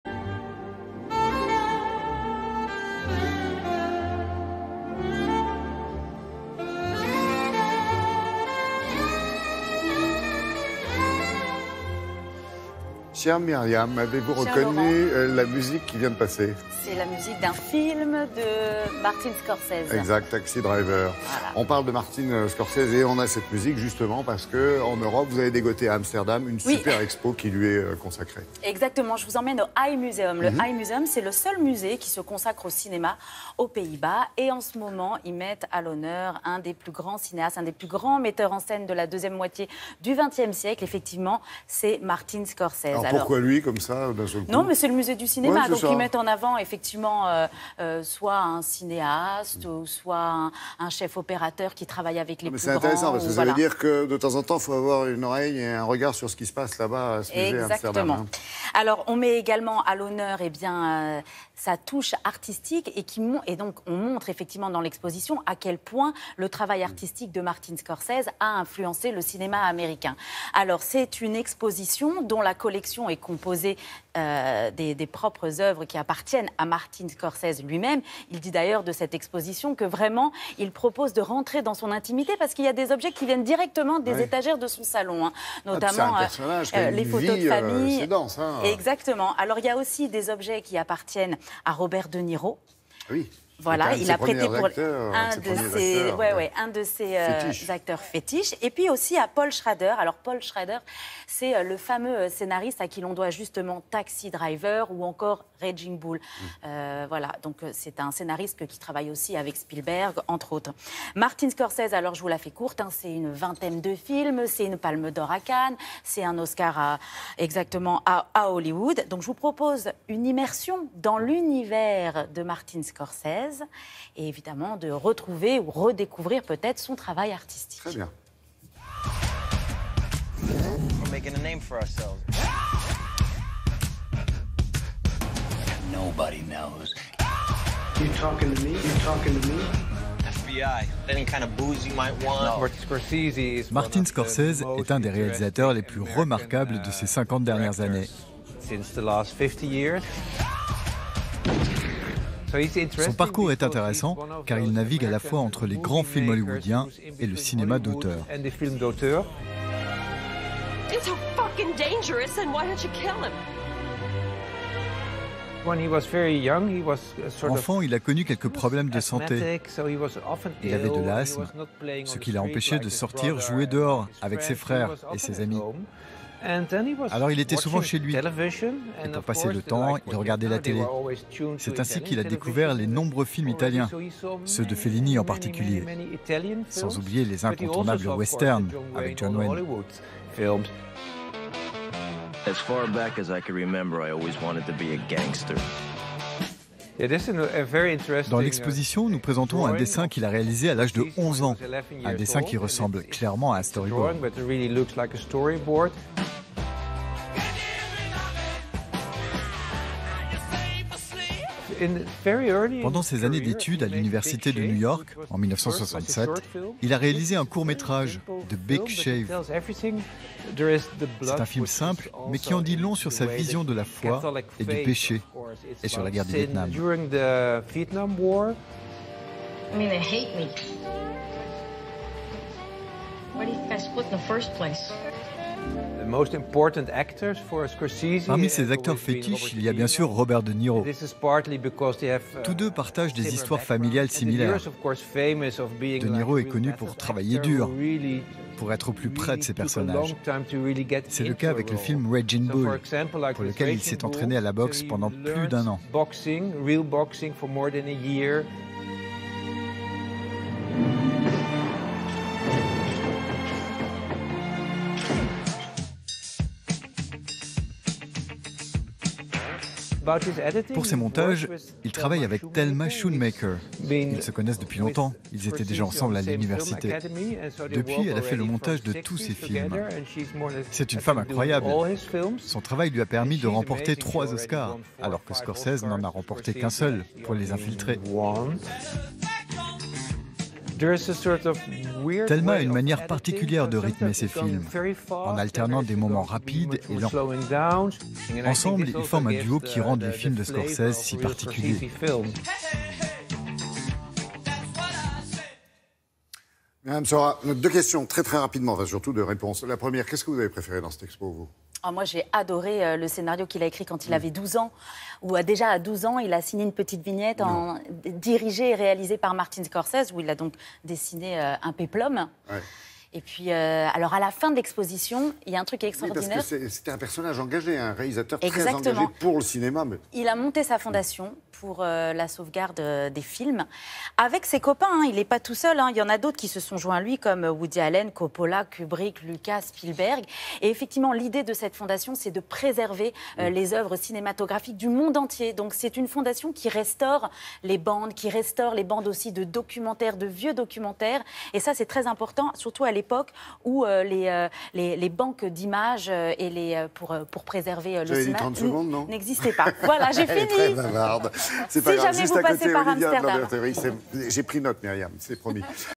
Europe - Amsterdam : Martin Scorsese à l’honneur
Jusqu’au 3 septembre 2017, le EYE Museum d’Amsterdam consacre une exposition au travail du réalisateur américain Martin Scorsese.